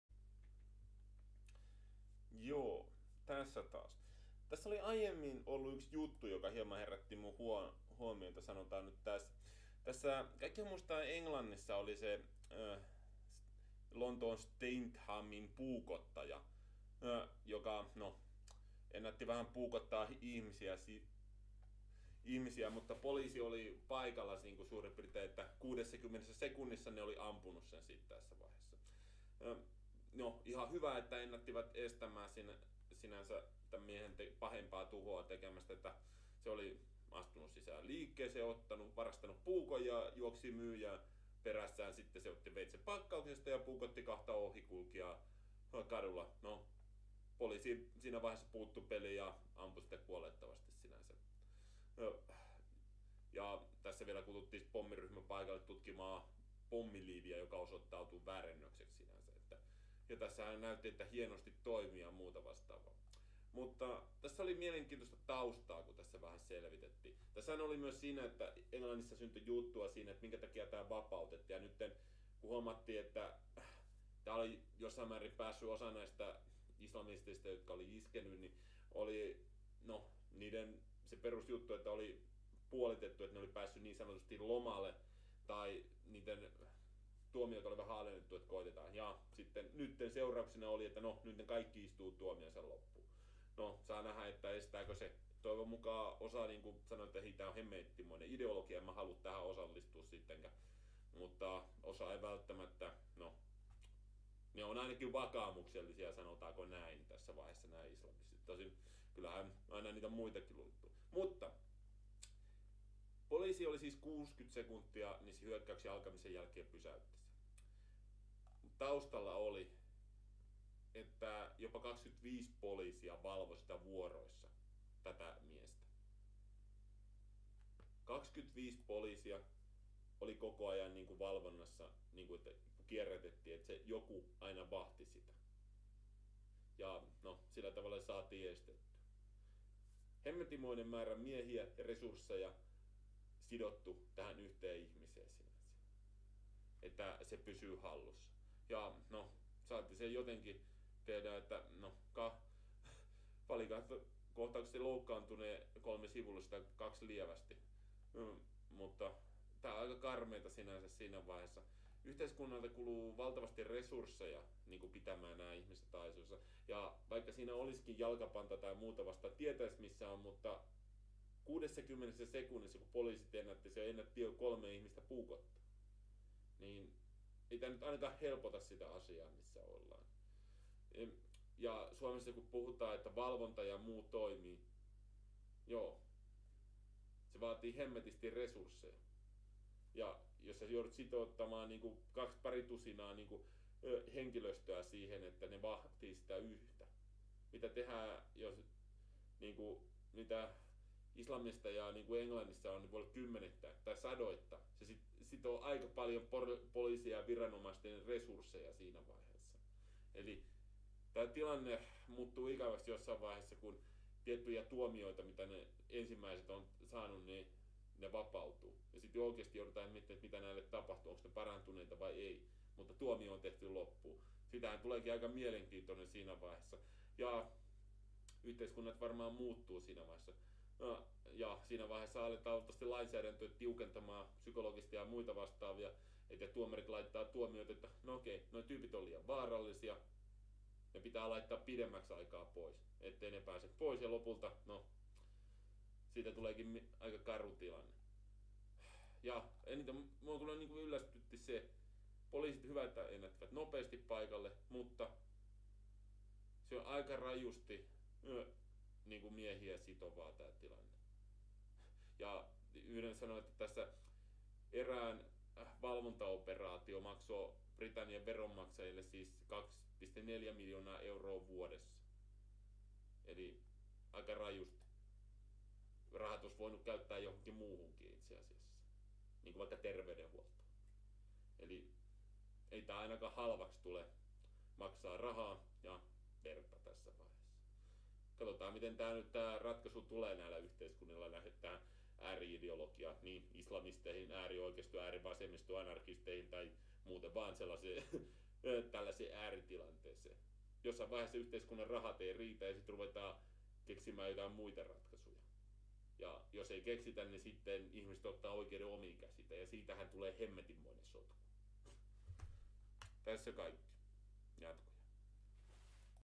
Islamistien valvonta ja pysäytys 60 sekunnissa?
Lontoon Streathamin puukottaja ennättä haavoittaa kolmea, ennenkuin viranomaiset pysäyttivät tämän.  Tämä oli jatkuvan valvonnan alla?  Yhteensä 25 SO15-ryhmän agenttia oli seurannut vuoroissa, tämä ei ole halpaa?